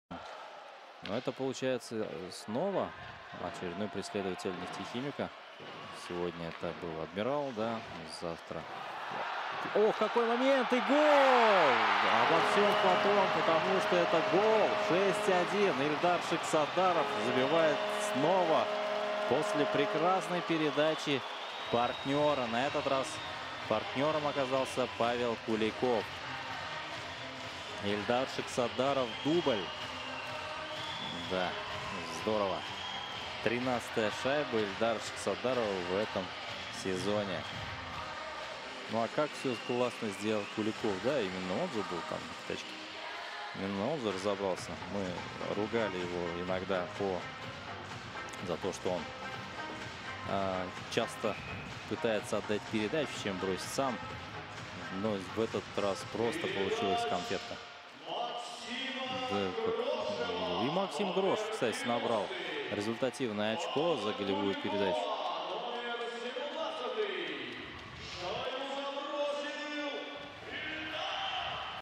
Kulikov sets up Shiksatdarov for lovely goal
Pavel Kulikov feeds Ildar Shiksatdarov for his second of the night

Павел Куликов чумовым пасом отправляет Шиксатдарова забивать